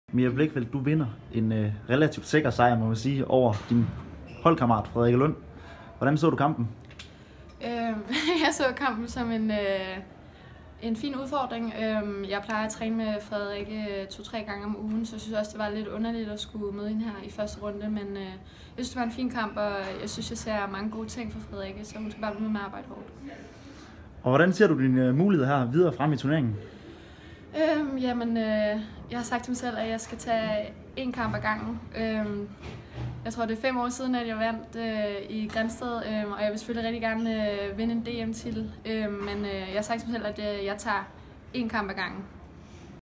Mia Blichfeldt: Jeg tager en kamp ad gangen
Mia Blichfeldt er videre til semifinalerne ved FZ FORZA/RSL DM 2019 efter en sejr over holdkammeraten Frederikke Lund 💪. Hun håber på gentage succesen fra danmarksmesterskaberne i 2014 🇩🇰🥳